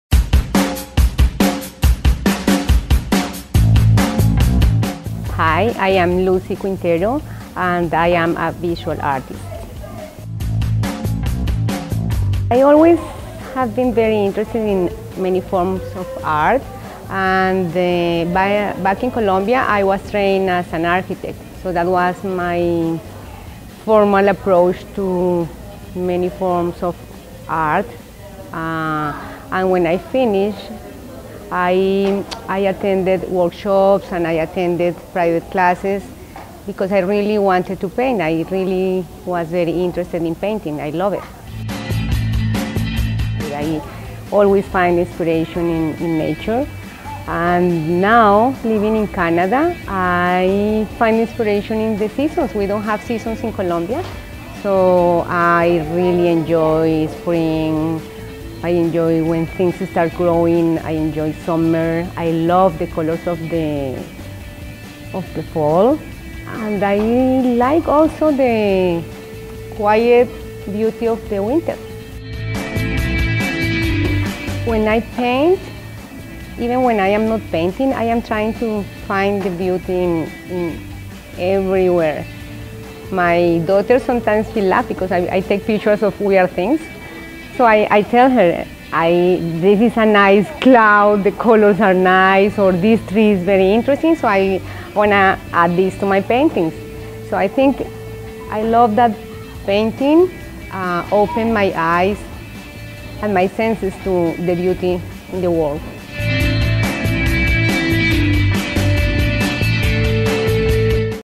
OPLW Featured Artist: Lucy Quintero
Visit Richmond Hill Public Library on Thursday, October 22 from 6-9pm and meet Lucy Quintero. She's one of more than half a dozen artists who will be live painting during our Ontario Public Library Week celebration.